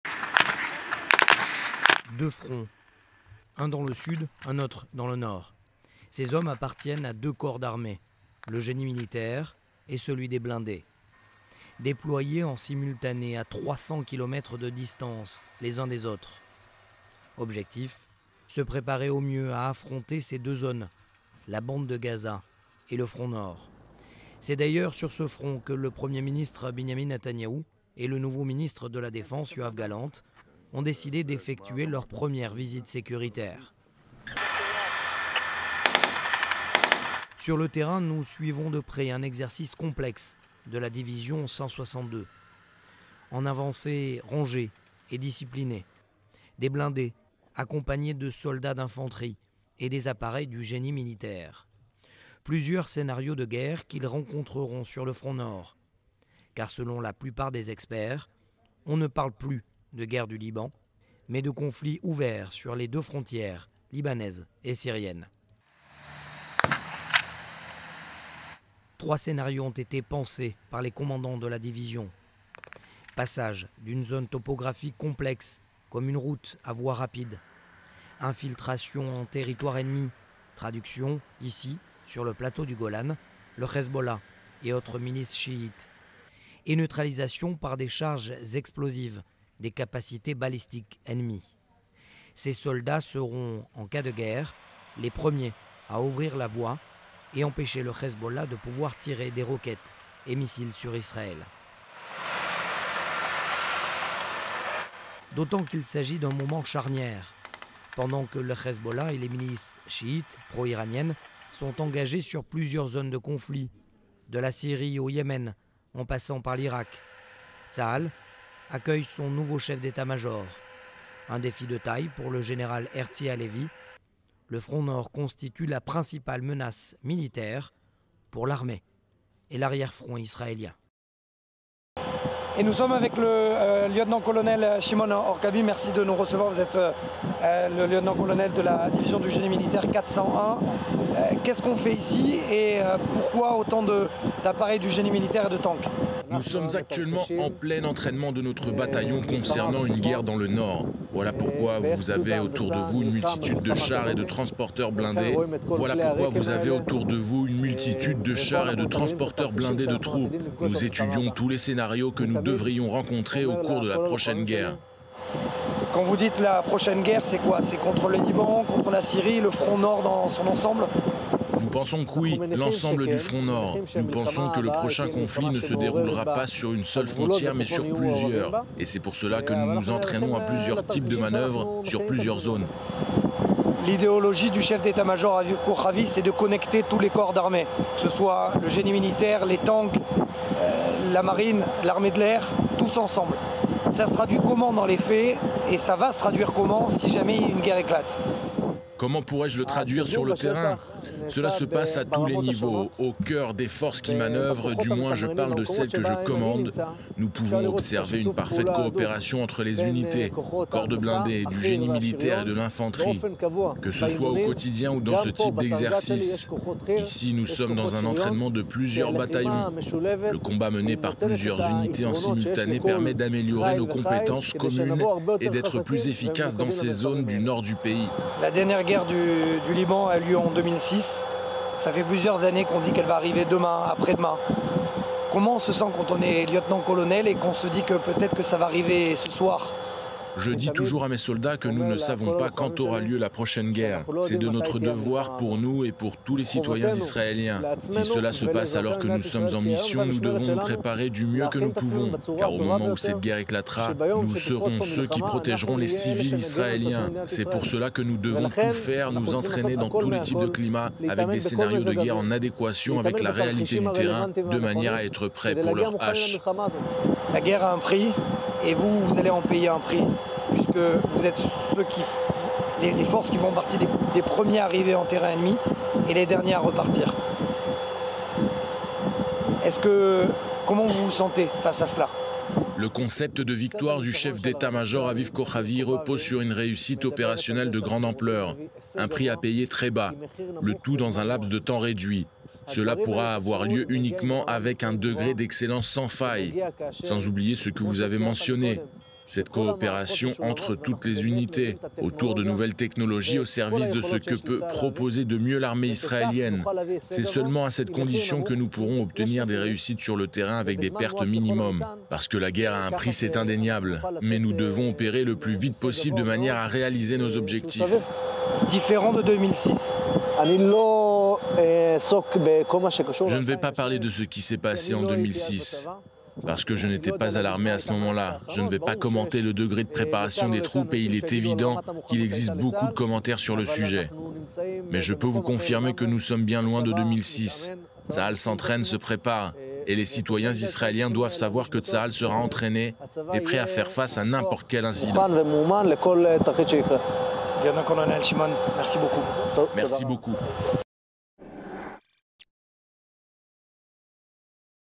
DÉFENSE | Israël : en immersion dans le bataillon 601 du corps des blindés
DÉFENSE | Le Lieutenant-colonel de l'armée israélienne Shimon Orkabi, commandant de bataillon 601, s'est entretenu avec Matthias Inbar au sujet des corps d'armée du génie militaire et des blindés. Ces derniers se préparent quotidiennement à faire face à des fronts ouverts sur deux frontières : le Liban et la Syrie.